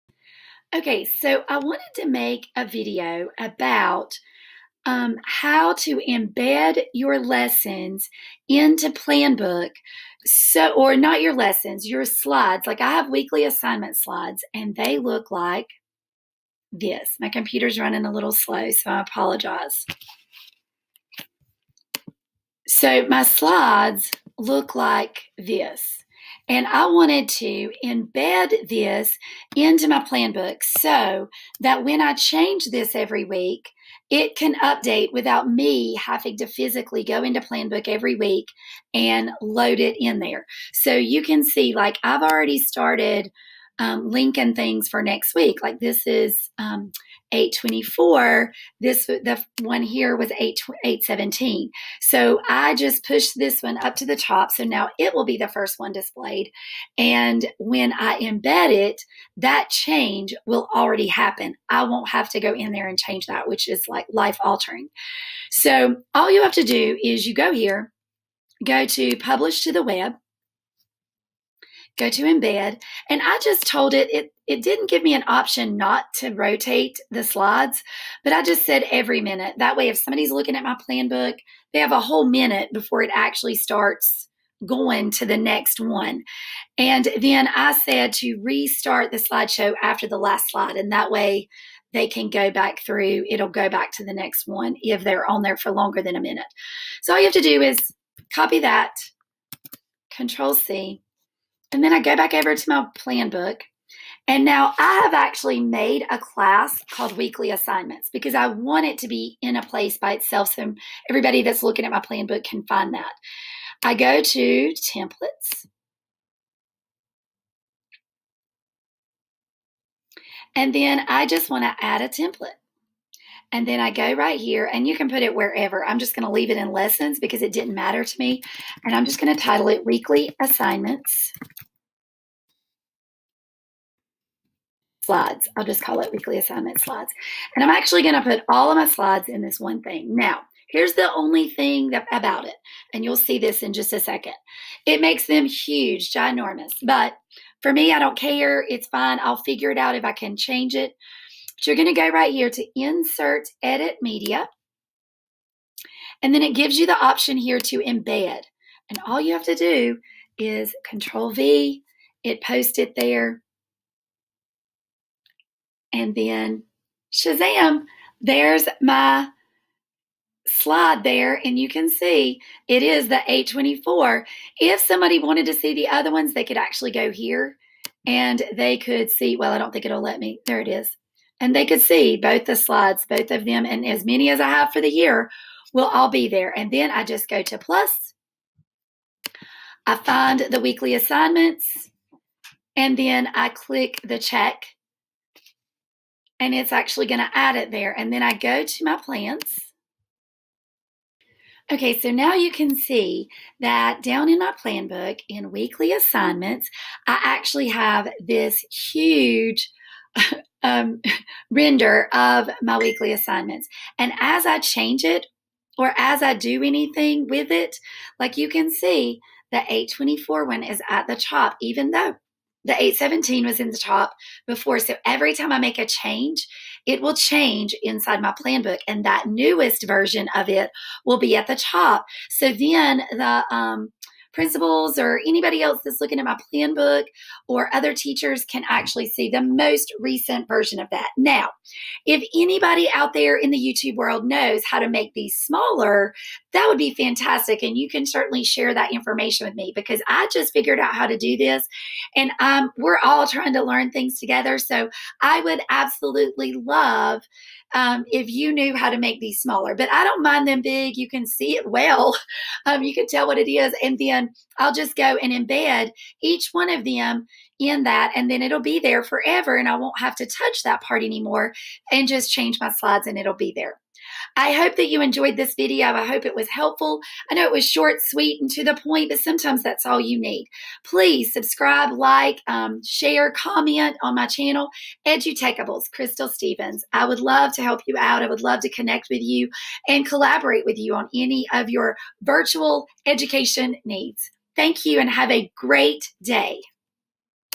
Embedding a google slide into Planbook.com
I made this video about how to embed a google slide into your planbook for easy editing.  You will no longer have to load them in every week or just have the link show up.  This actually embeds the image of your weekly assignment slides (or anything you may want to embed) into Planbook.com so that when you make changes to the document -- it will automatically change the planbook image.  YAY work smarter not harder.